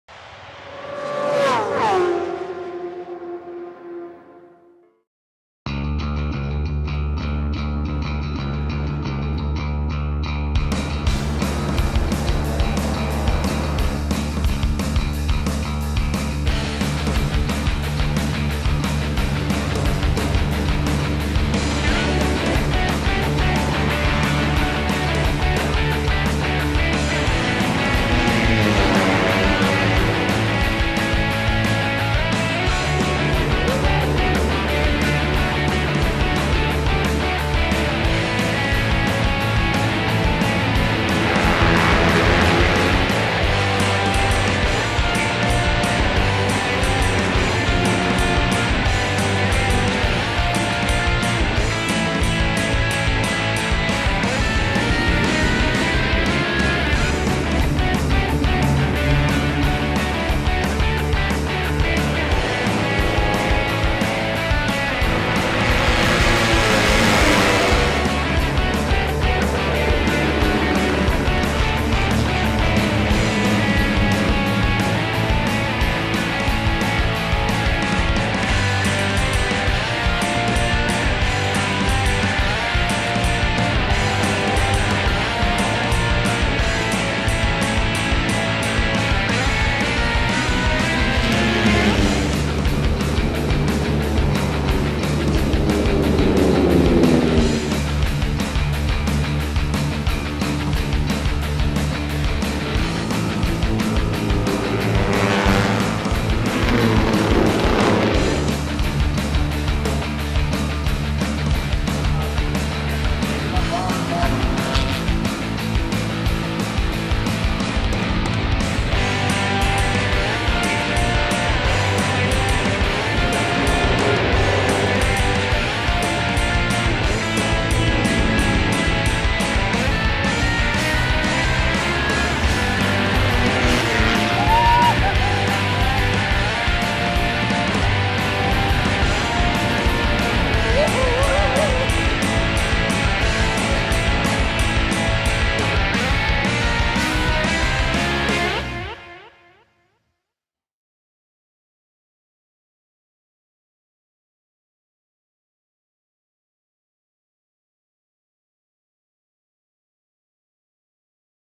Best Of - Sandro Cortese
He may have had an incredibly consistent season as far as results go on his way to the Moto3™ title in 2012, but Sandro Cortese was involved in some terrific battles along the way, as well as a few controversies. Take a look at the "Best Of" Sandro Cortese.